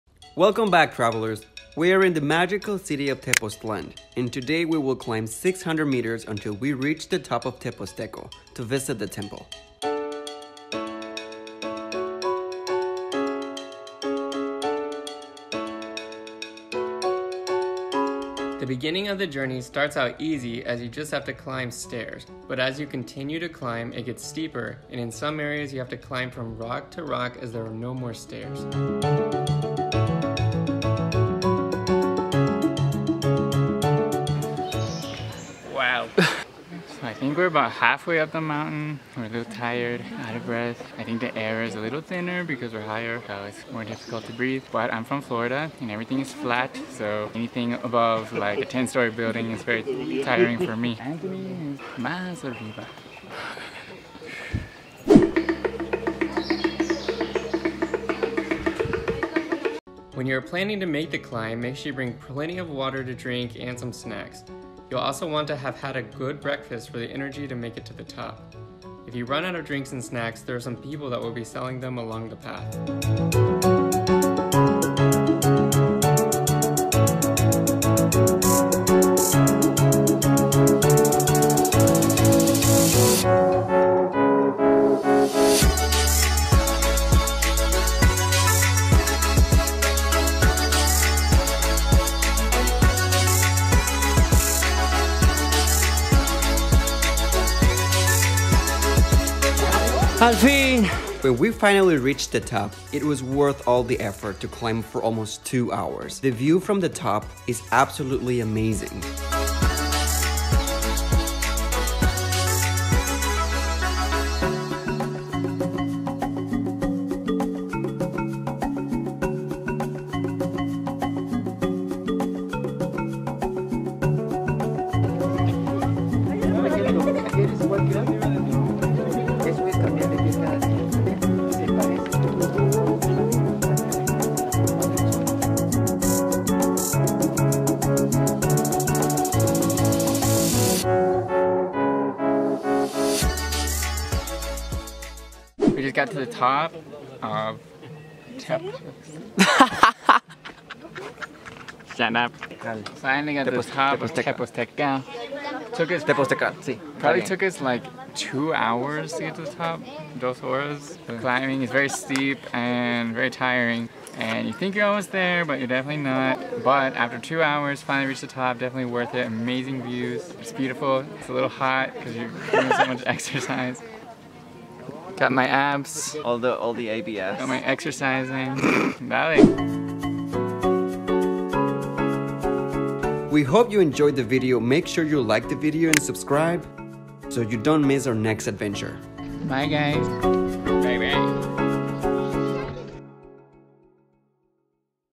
Magical Mountains in TEPOZTLAN MEXICO
Welcome back travelers. Today we took a bus from Mexico City into the City of Tepoztlán to climb to the top of the magical mountain of Tepozteco. The climb took about two hours for us, stopping for some photos along the way. At the top you can get absolutely breathtaking views of the surrounding area. The mountains here look like they are floating and is unlike anything we’ve ever seen before. Back in the city there are plenty of shops to buy suvioneers and grab a bite for lunch.